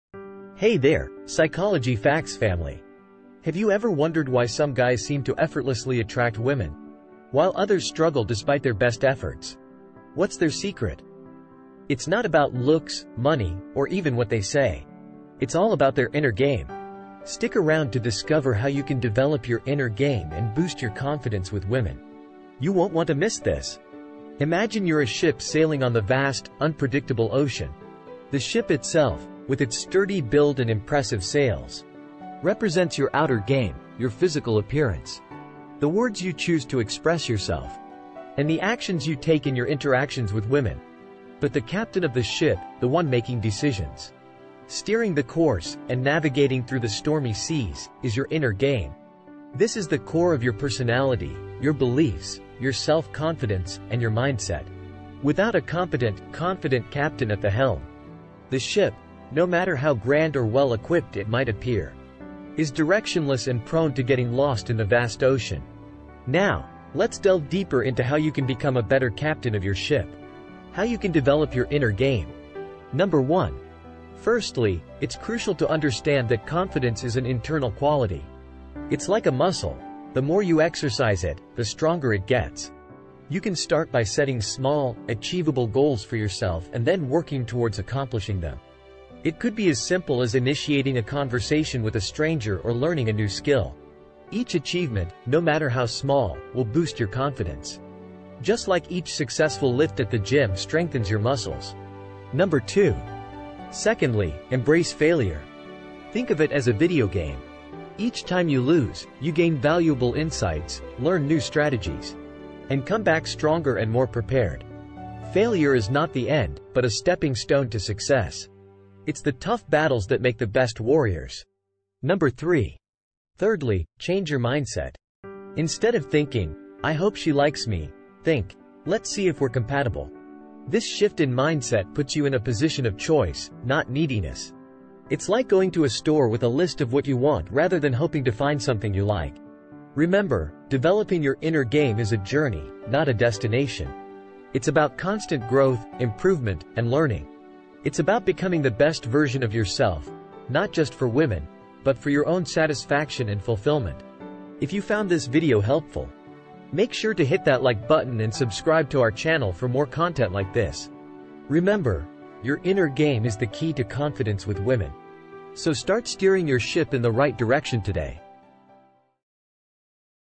Master Your Inner Game and Unleash the Power of Attraction
Are you struggling to find love or feel stuck in your relationships? Do you feel like you have no control over your own life? Do you feel like you're just not good enough?

In this video, we're going to reveal the secret to unlocking the secret of attraction. This step-by-step guide will teach you how to master your inner game and attract the person or situation you want in your life.

If you're looking for ways to improve your self-esteem, learn how to be more social, or tackle mental health issues, then this is the video for you! By following the steps in this video, you'll be on your way to becoming the person you've always wanted to be.